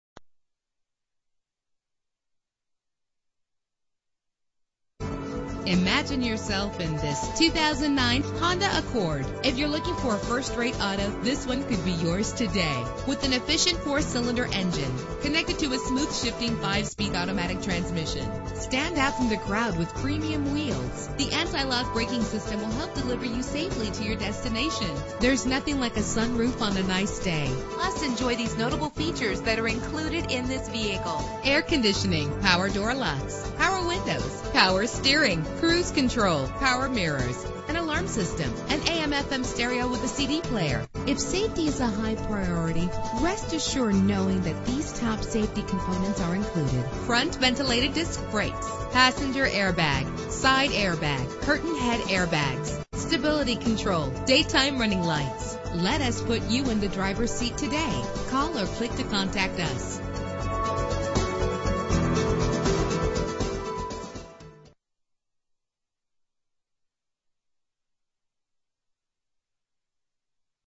2009 Honda Accord - Burlington NJ
Year: 2009
Make: Honda
Model: Accord
Trim: 2.4 EX
Engine: 2.4 liter inline 4 cylinder 16 valve
Transmission: 5-Speed Automatic
Color: Alabaster Silver Metallic
Mileage: 34018
Address:
4397 Rt 130 S